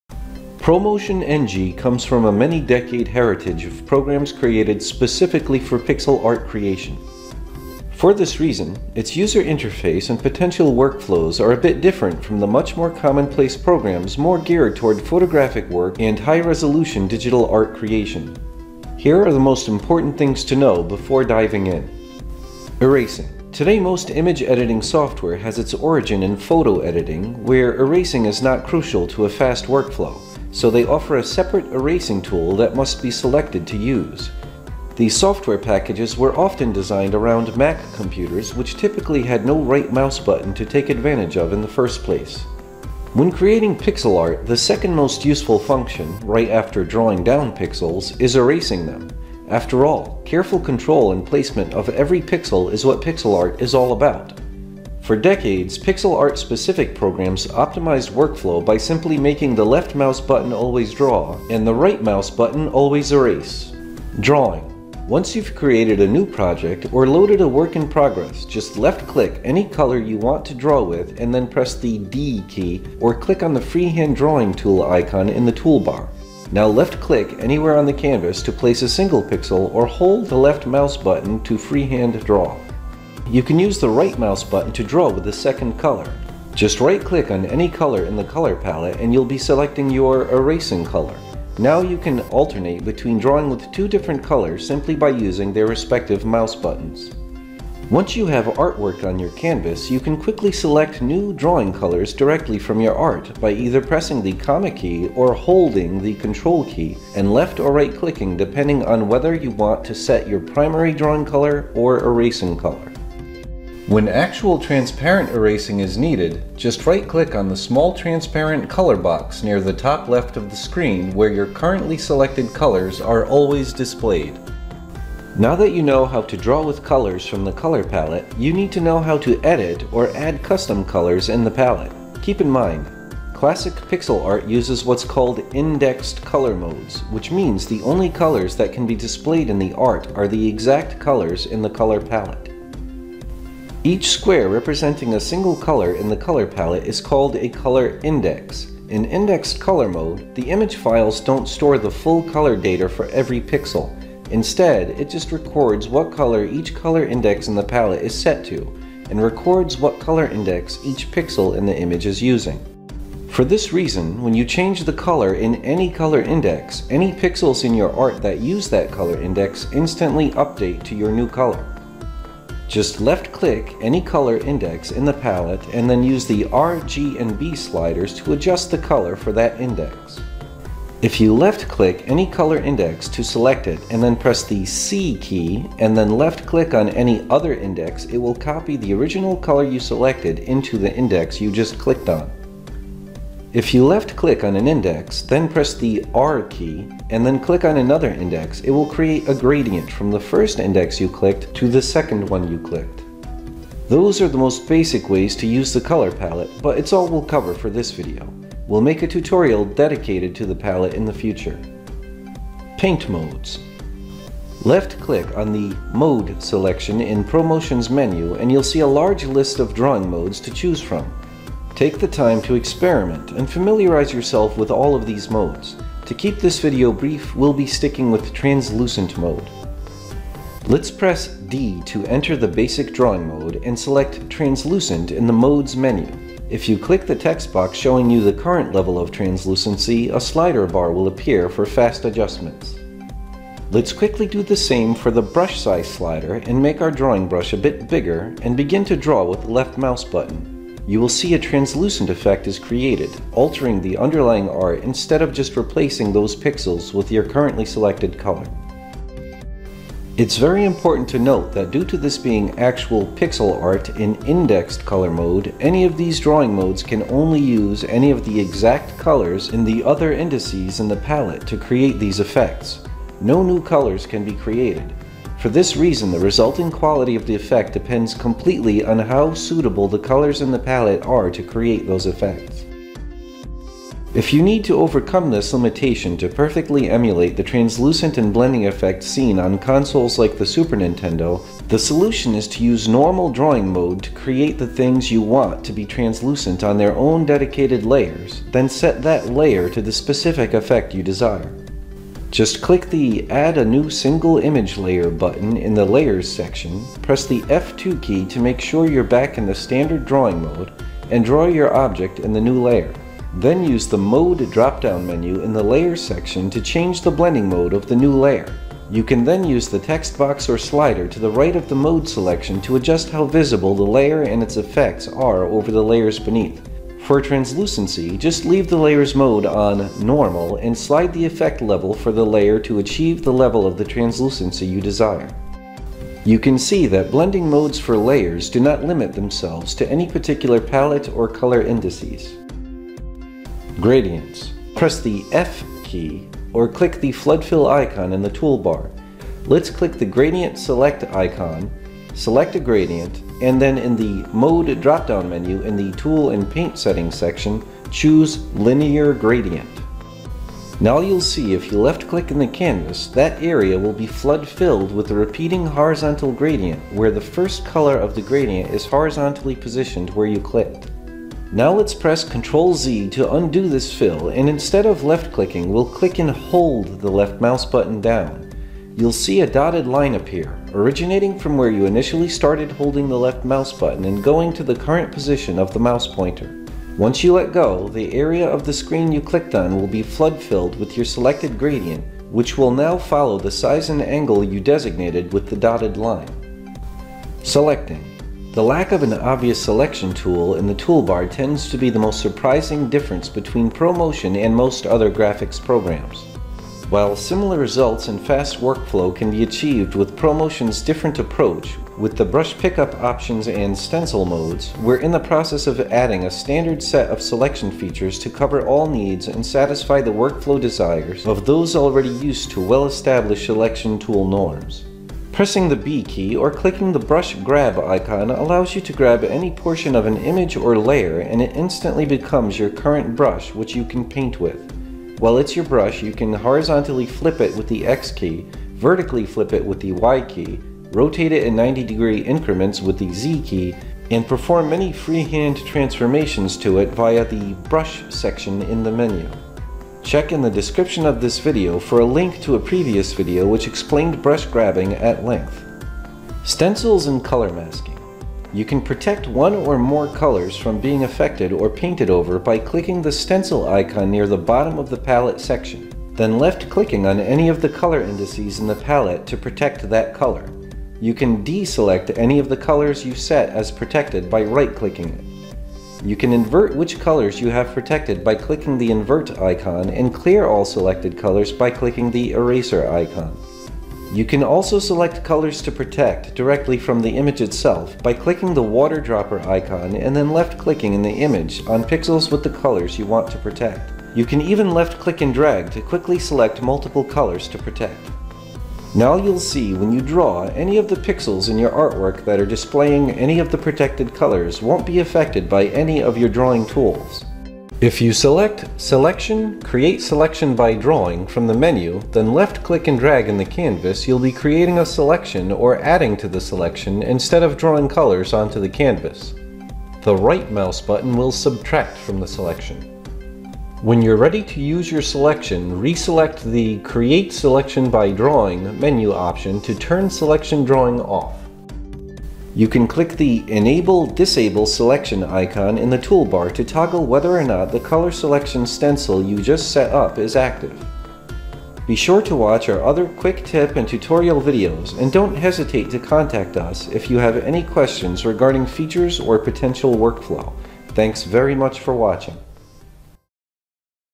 Intro to using Pro Motion NG
This video helps get you familiar with Pro Motions most core features and workflow.